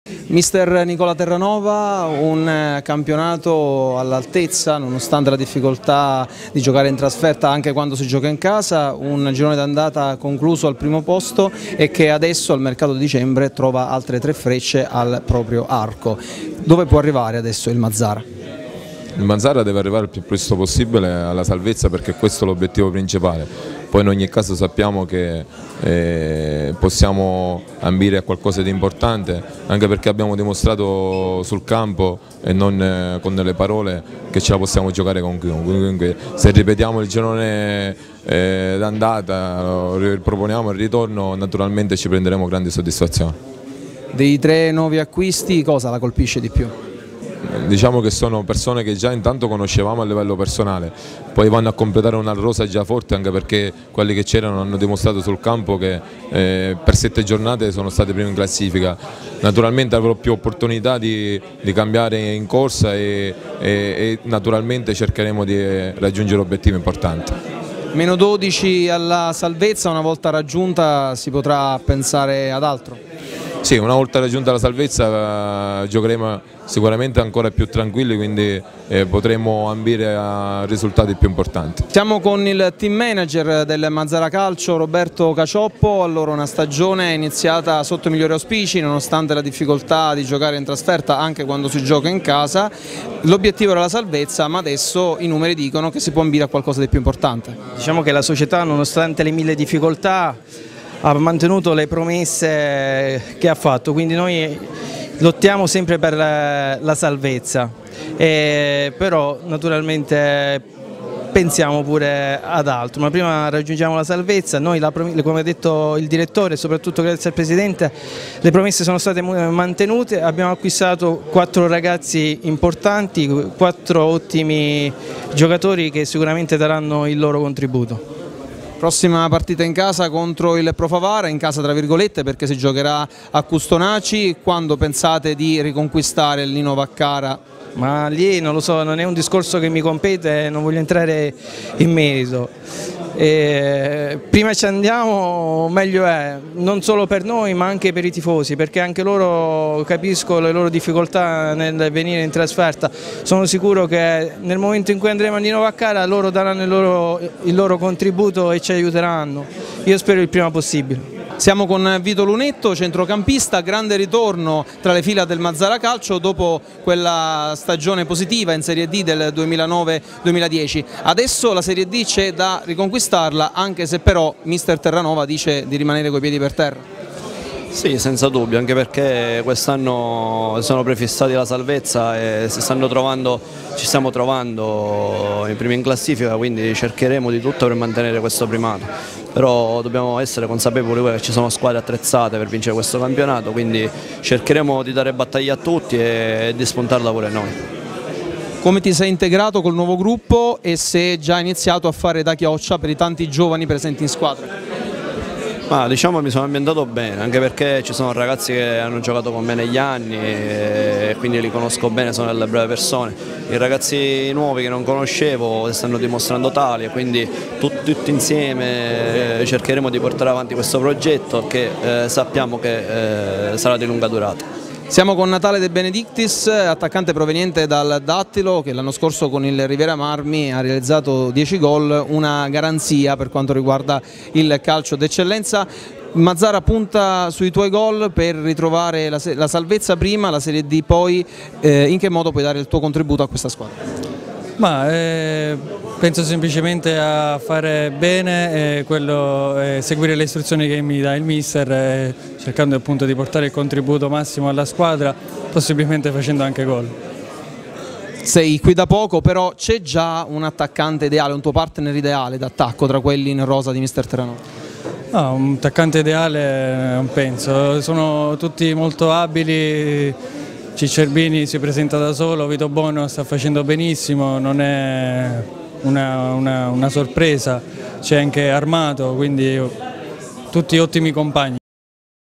2014-12-16 Conferenza stampa Mazara Calcio, nuovi acquisti canarini
Conferenza stampa Mazara Calcio, nuovi acquisti canarini. Servizio di Tommaso Ardagna